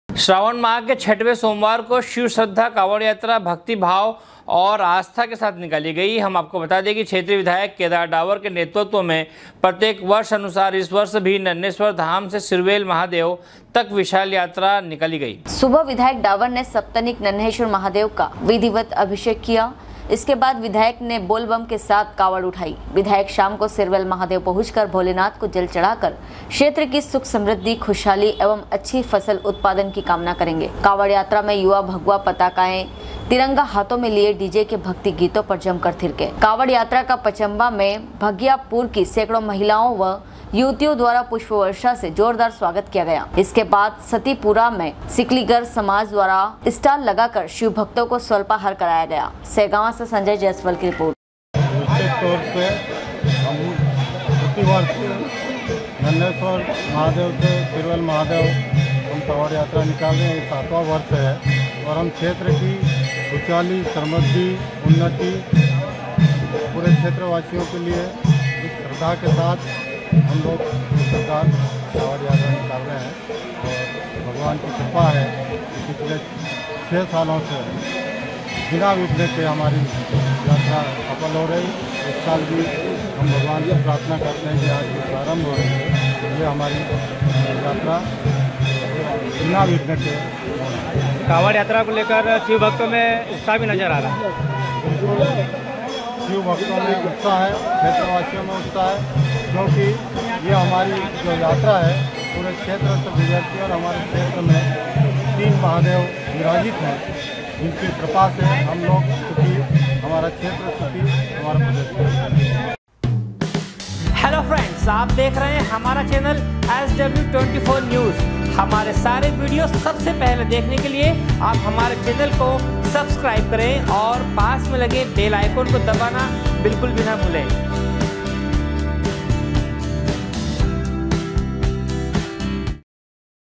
श्रावण माह के 6 वे सोमवार को शिव श्रद्धा कावड़ यात्रा भक्ति भाव और आस्था के साथ निकाली गईं।
श्रावण माह के 6 वे सोमवार को शिव श्रद्धा कावड़ यात्रा भक्ति भाव और आस्था के साथ निकाली गईं। बता दे कि क्षेत्रीय विधायक केदार डावर के नेतृत्व में प्रत्येक वर्ष अनुसार इस वर्ष भी नन्हेंश्वर धाम से सिरवेल महादेव तक विशाल कावड़ यात्रा निकाली गईं।